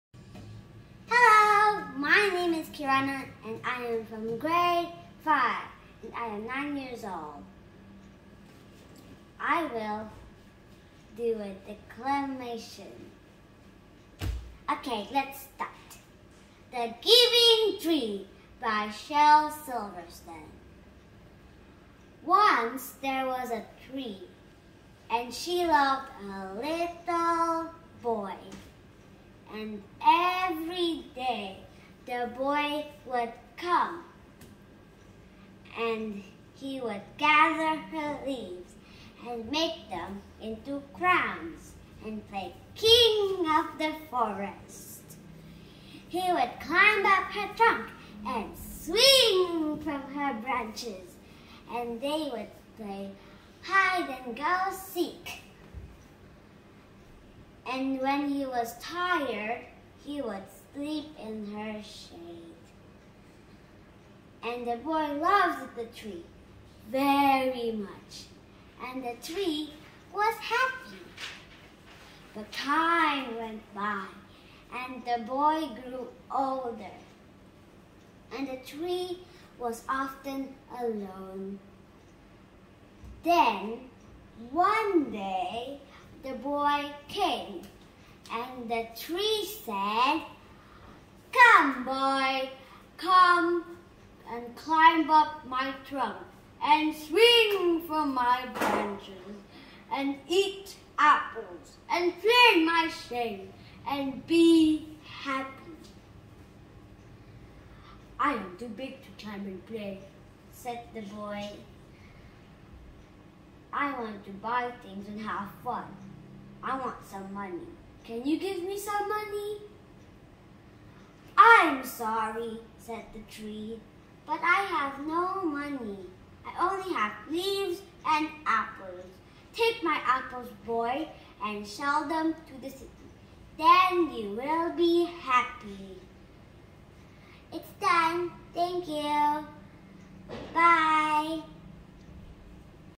Some parts of 'The Giving Tree' by Shel Silverstein..Declamation by Kirana (9Yo)
Tugas sekolah😊